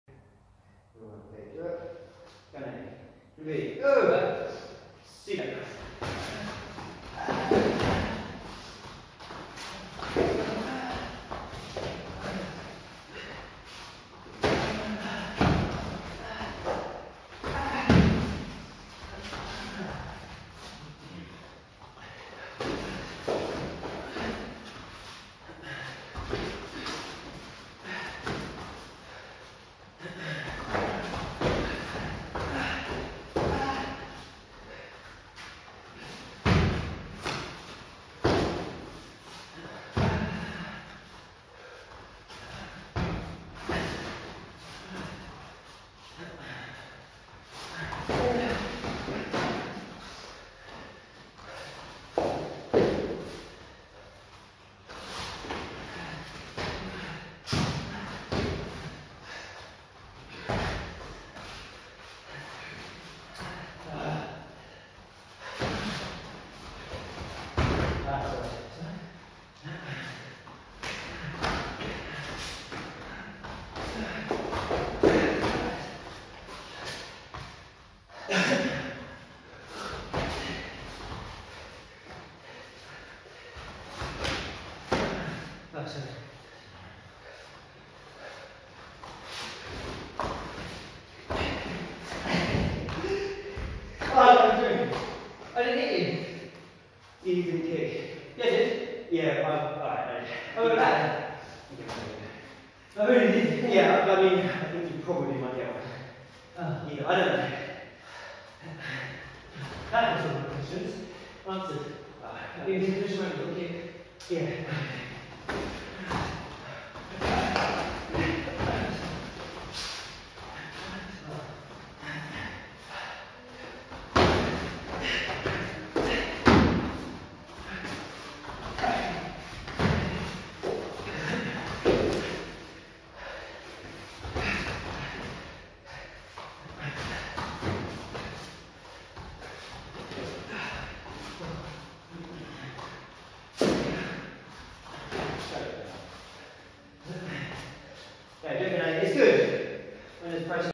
Taekwondo Sparring Training- Dean Magee and Master Luke Robinson.
Am extra hour and a half private class after 3 hours of sparring training.